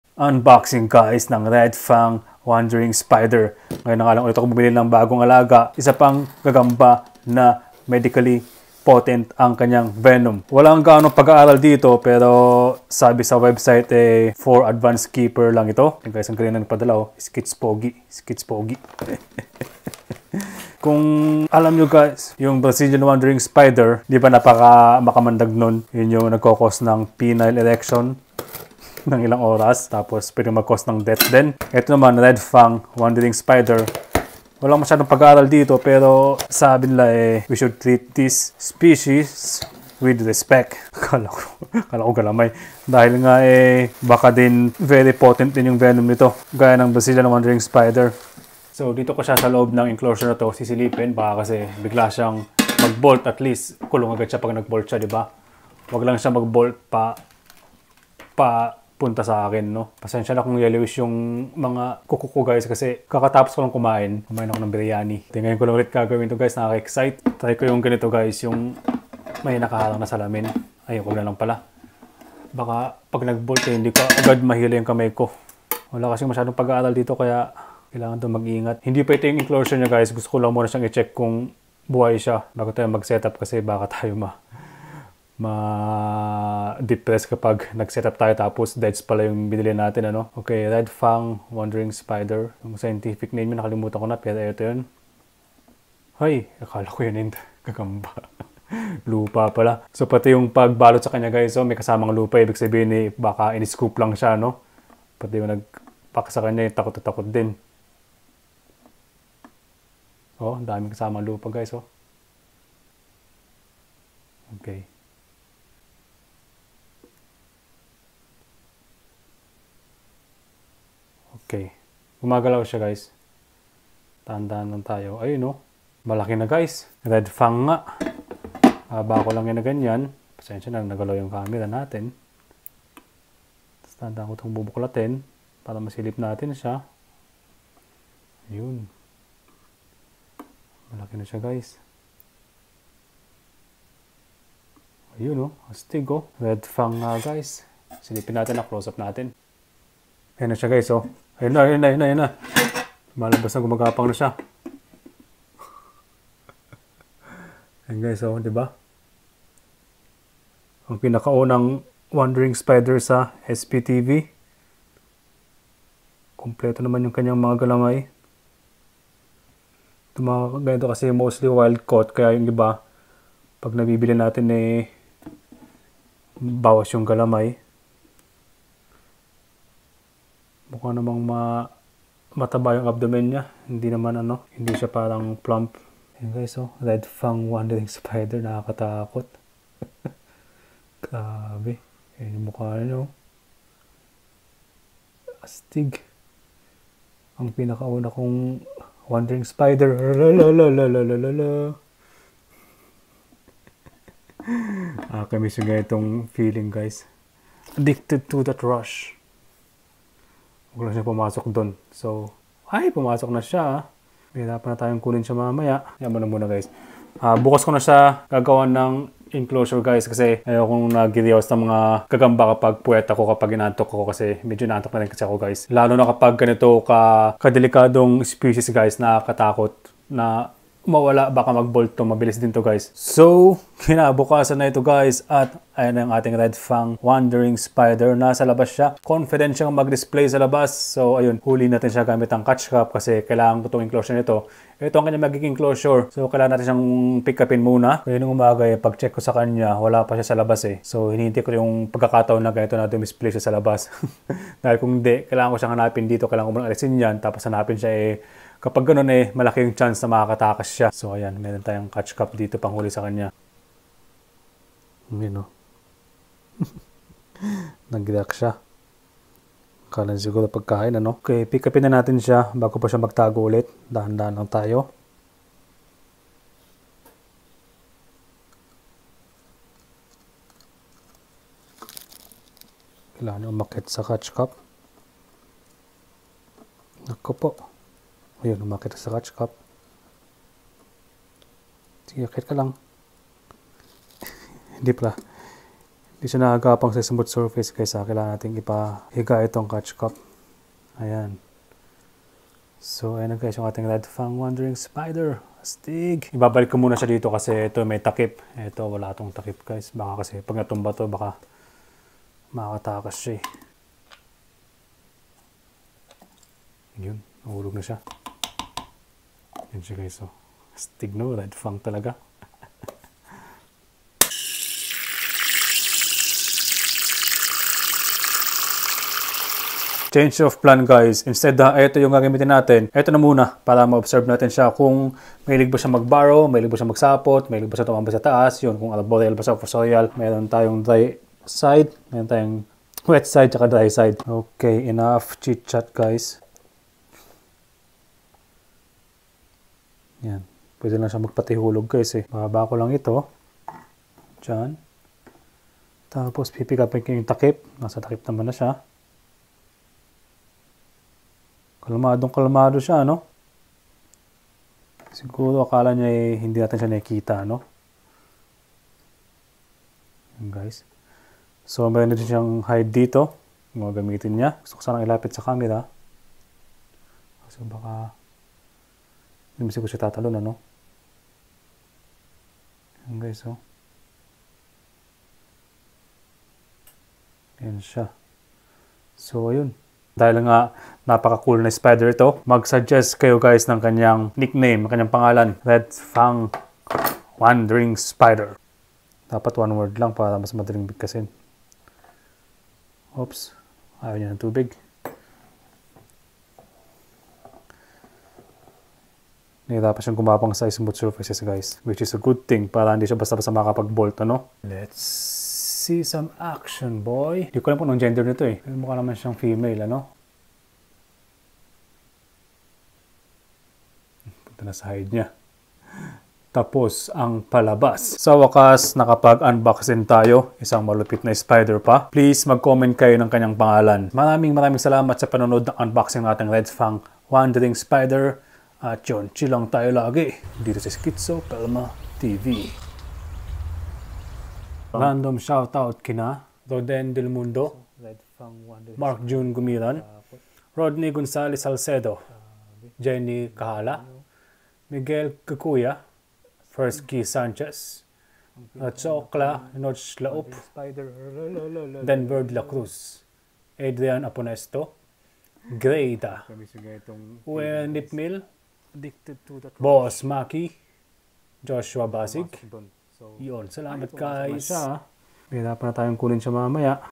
Red Fang Wandering Spider Unboxing!
Ngayon na nga lang ulit magu-unbox, Wandering Spider pa! 🫣 Samahan niyo ako mag-unbox ng Red Fang Wandering Spider! Ang Piloctenus haematostoma, very potent din kaya venom nito gaya ng Brazilian Wandering Spider? Addicted To That Rush ulit...